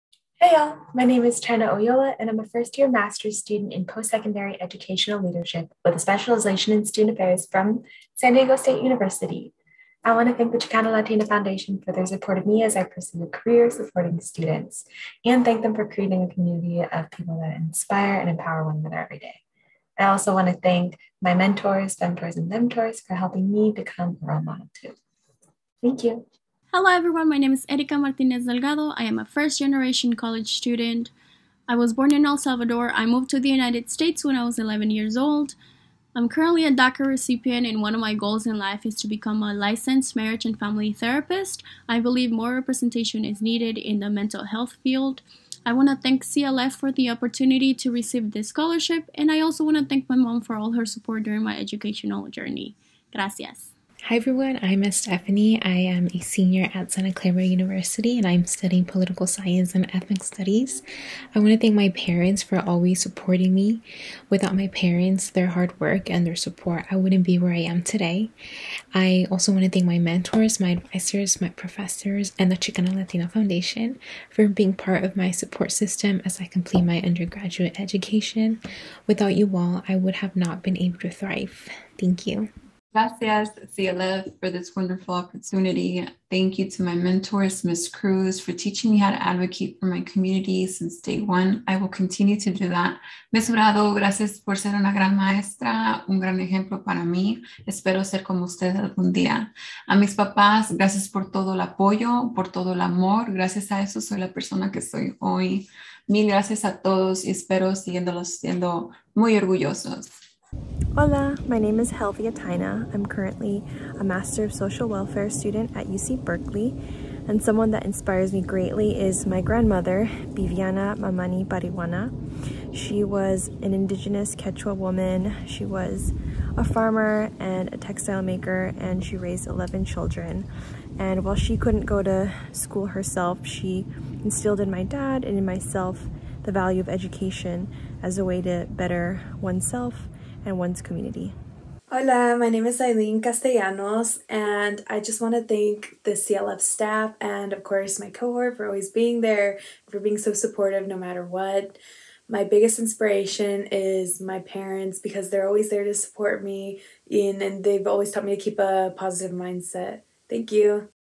2021 Cohort (Part 2) - CLF Annual Awards Celebration 2021
1. Chyna Oyola
2. Erika Martinez Delgado
3. Estefany Meza
4. Fanny Camacho Medina
5. Helvia Taina
6. Ilyne Castellanos